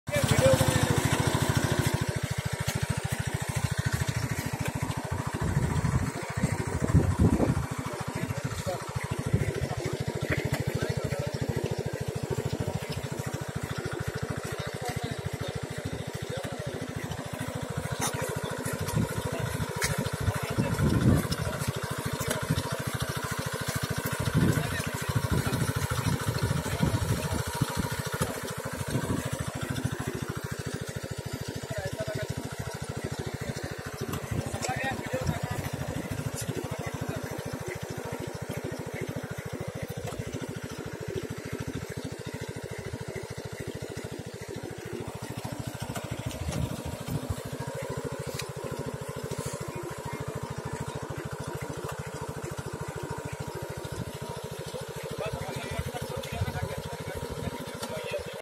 धान की मशीन से रोपाई
Paddy nursery transplanting successfully done by VST with collaboration of KVK Gondia District of Maharashtra.